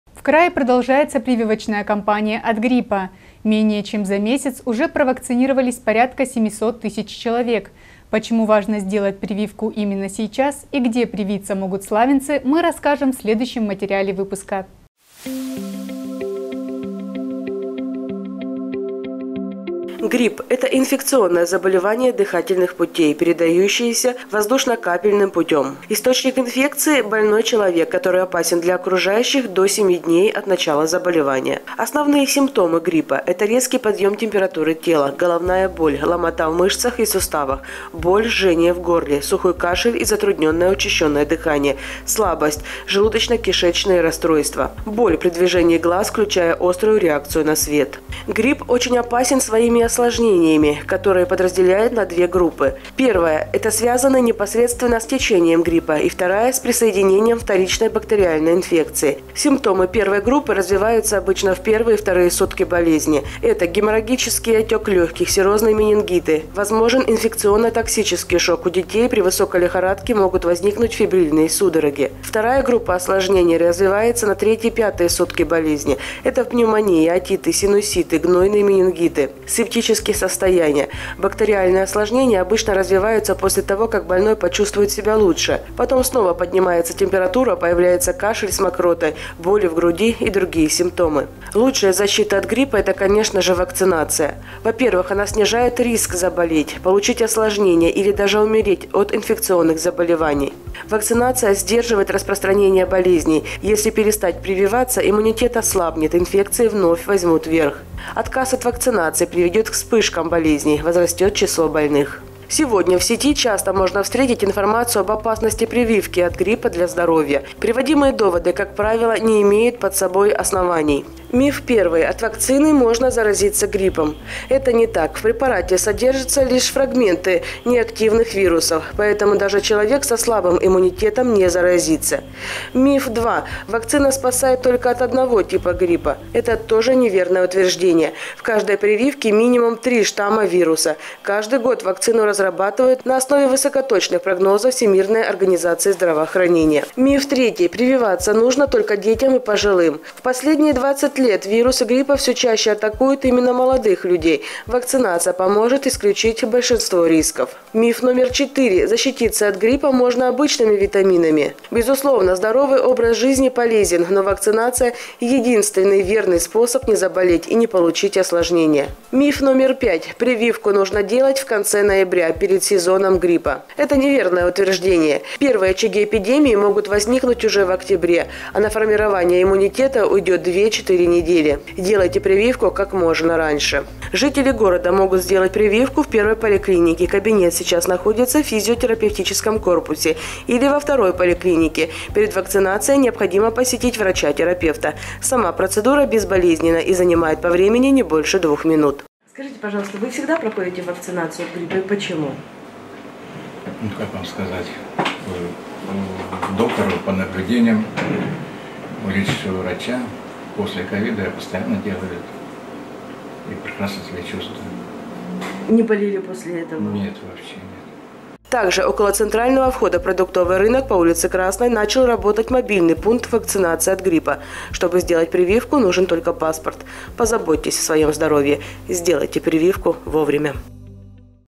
Время прививаться
В крае продолжается прививочная кампания от гриппа. Менее чем за месяц уже провакцинировались порядка 700 тысяч человек.  Почему важно сделать прививку именно сейчас и где привиться могут славянцы, мы расскажем в следующем материале выпуска